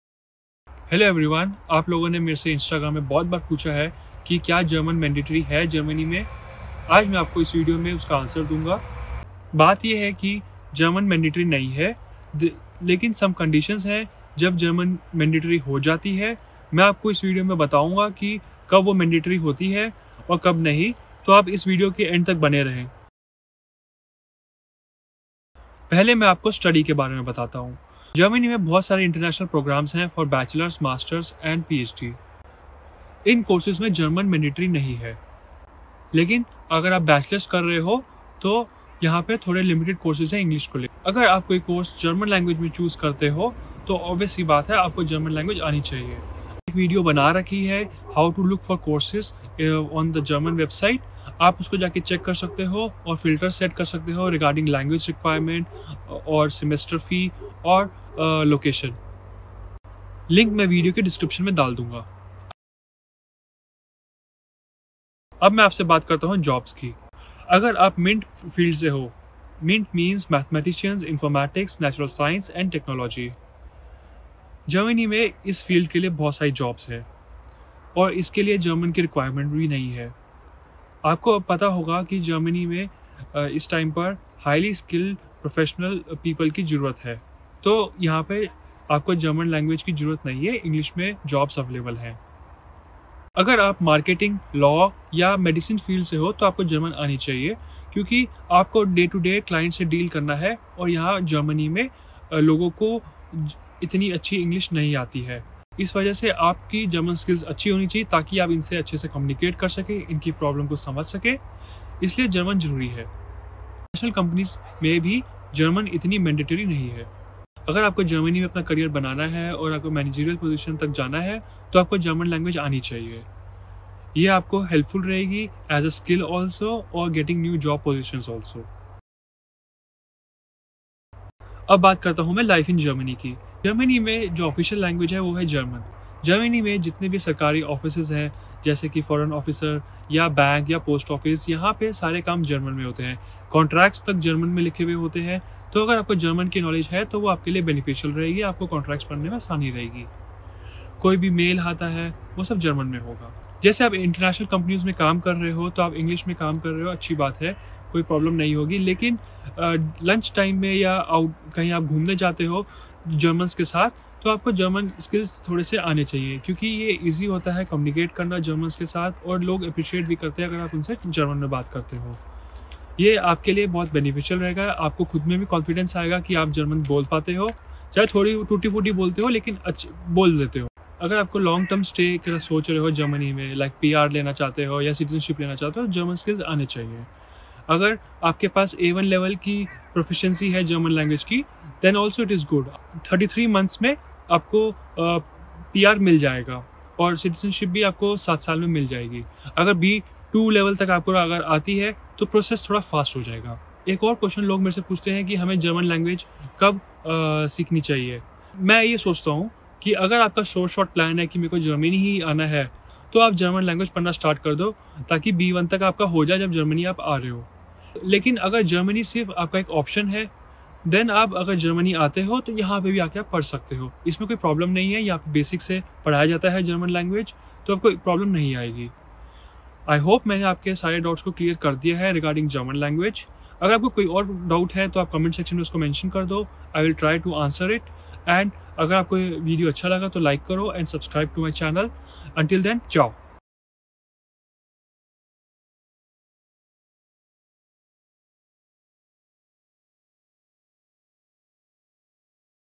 Kya German Mandatory hai Germany mei? | Study in Germany | Work in Germany | Hindi | हिन्दी
Hello Everyone, in this video I have provided detailed description about when German is mandatory and when it is not.

--- More Links---